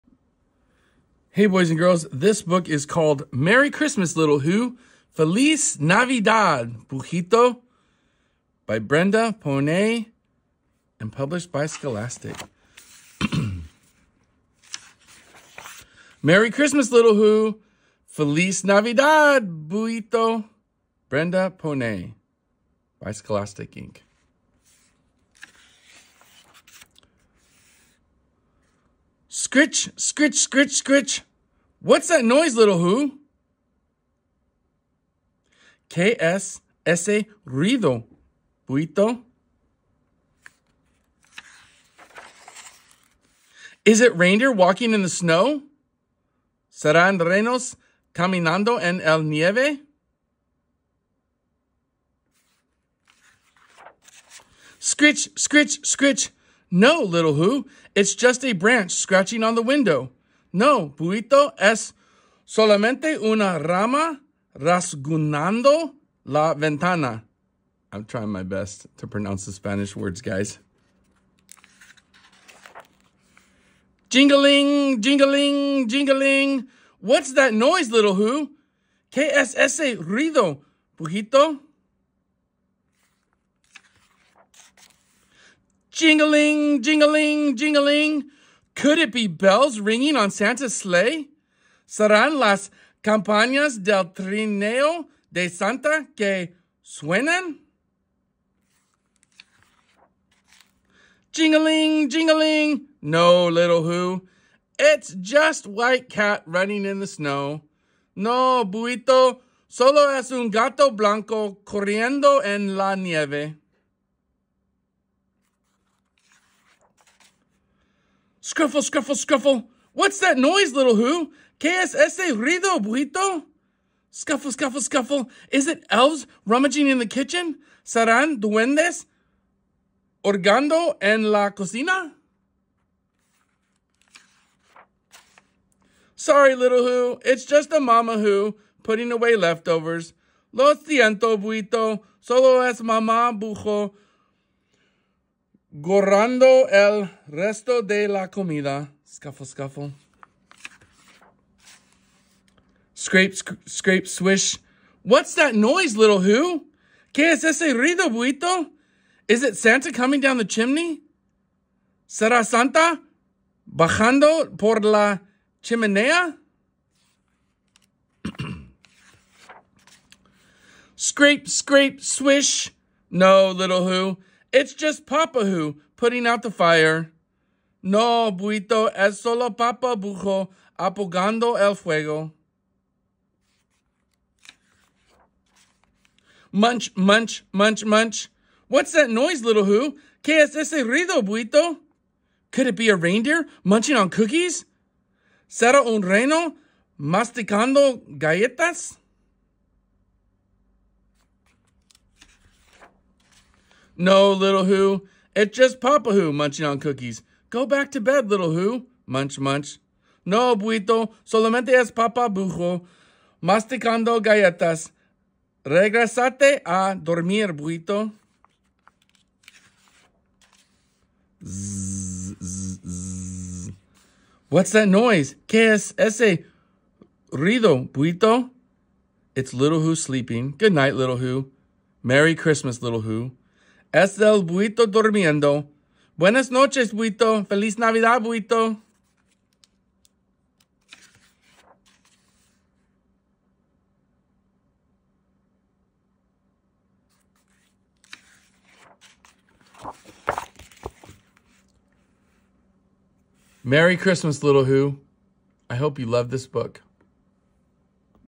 Merry Christmas Little Hoo! by Brenda Ponnay for Scholastic is a read aloud in Spanish and English
This is a fun book that has words in both Spanish and English. It tells the story of a little owl that is waiting for Santa.